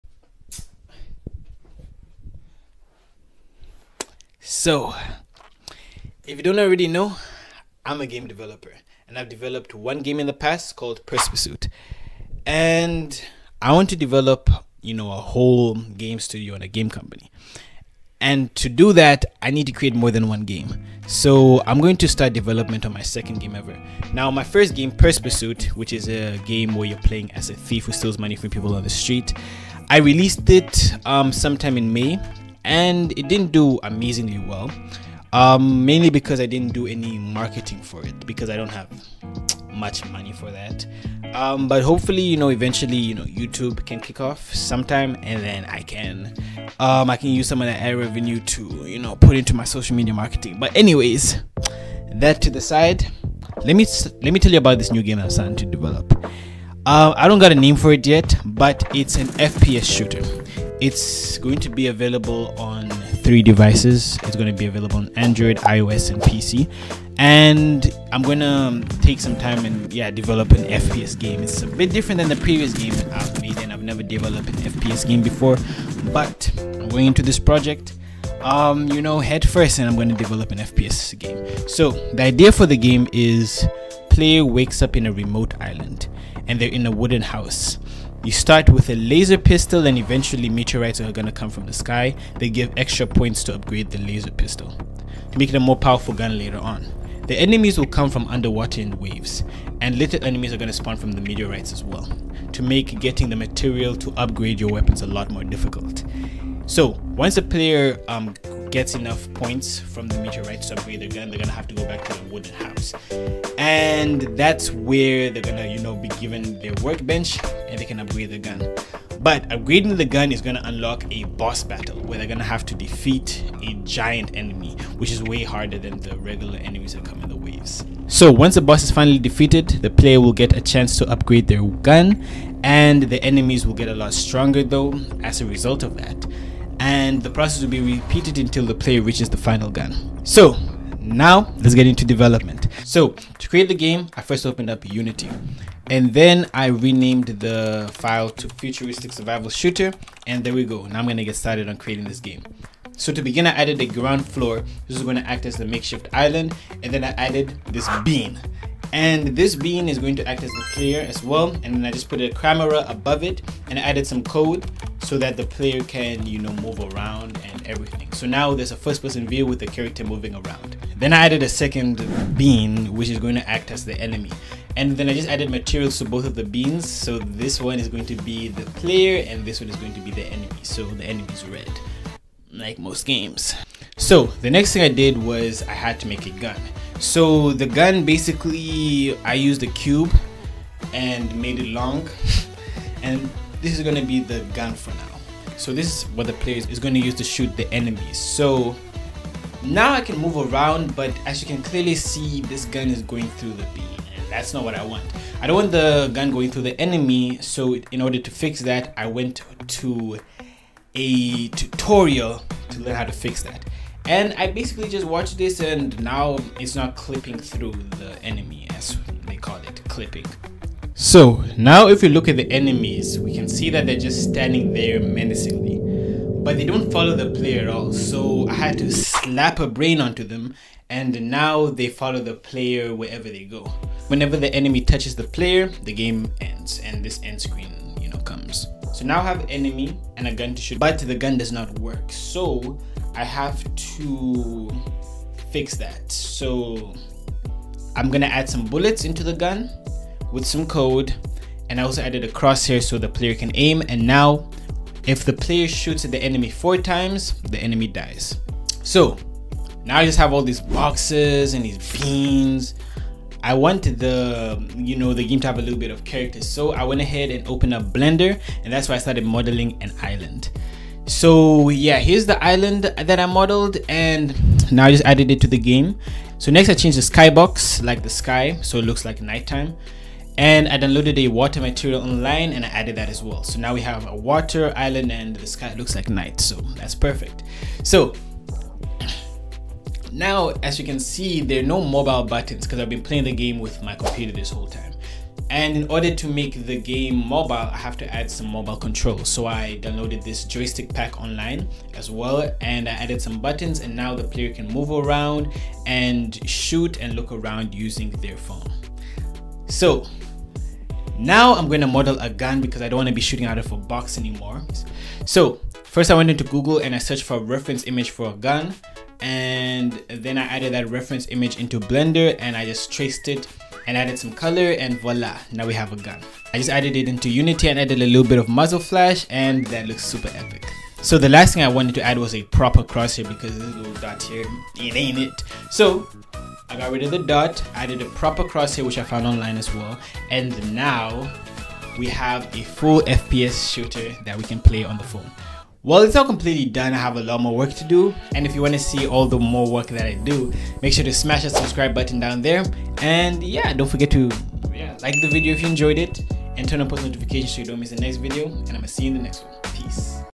I'm DEVELOPING an FPS GAME | Devlog 1
I'm making a mobile FPS game with Unity and C#. This is the first video in this series, enjoy! The game is based on a remote island where the enemy has a pistol; their aim is to kill enemies and collect resources from meteorites to upgrade their pistol until they unlock the final weapon.